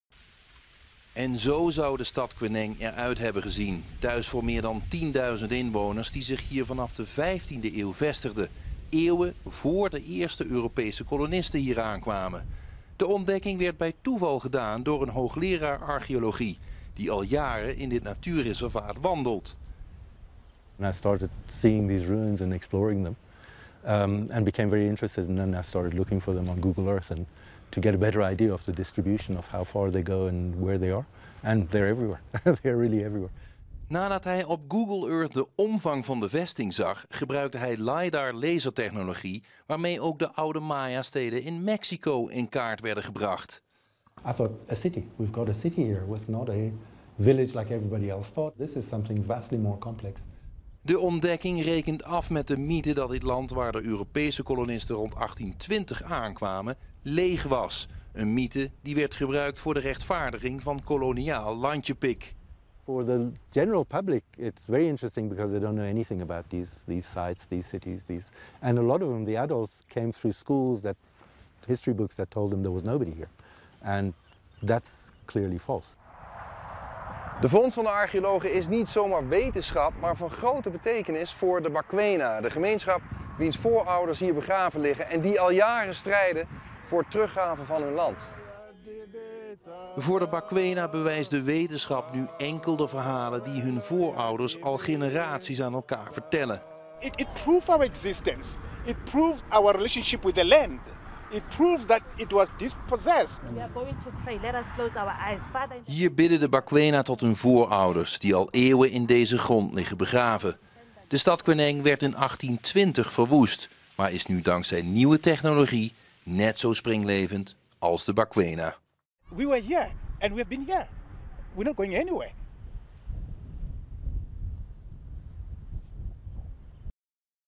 ZUID-AFRIKA: Stad uit 15e eeuw ontdekt in Suikerbosrand, bij Johannesburg
Professor Karim Sadr van de Witwatersrand Universiteit stuitte hij jaren geleden in het natuurreservaat Suikerbosrand, bij Johannesburg, op hopen stenen die veel weg hadden van eeuwenoude nederzettingen. Pas toen hij het gebied vanuit de ruimte ging bekijken, met Google Earth en een lasertechnologie die LiDAR heet, begon hem te dagen: "Hier lag een hele stad".

De eerste bebouwing van het gebied blijkt uit de 15de eeuw te dateren, ruim vier eeuwen voor de eerste Europeanen arriveerden in het gebied dat nu Johannesburg heet. "Ik realiseerde me niet alleen hoe uitgebreid de stad was, maar ook hoe welvarend", zegt Sadr. Hij noemt de stad Kweneng, de hoofdstad van de Tswana-gemeenschap.

De ontdekking is groot nieuws onder de kenners van de geschiedenis van zuidelijk Afrika. "Dit verandert alles", zegt de directeur van het Afrika-Studiecentrum in Leiden, Jan-Bart Gewald. Dat heeft te maken met de koloniale vertelling over het gebied rondom Johannesburg. Volgens die vertelling troffen de eerste Europese kolonisten die in 1836 daar aankwamen een leeg land aan, met hier en daar wat nomaden en kleine huishoudens. Die vertelling rechtvaardigde koloniale wetten, zoals de Landwet uit 1913, en wetten uitgevaardigd door het apartheidsregime (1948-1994), die ruim 3,5 miljoen Zuid-Afrikanen van hun land verjoegen.

"Veel Zuid-Afrikanen is geleerd dat er niemand was hier. En dat is overduidelijk onwaar", zegt Sadr, uitkijkend over een nu overgroeid gebied dat hij beschrijft als "het centrum van een grote stad". De stad werd in 1820 verwoest in een burgeroorlog die bekend staat als de Difeqane.

► VRAGEN/TIPS/IDEEËN?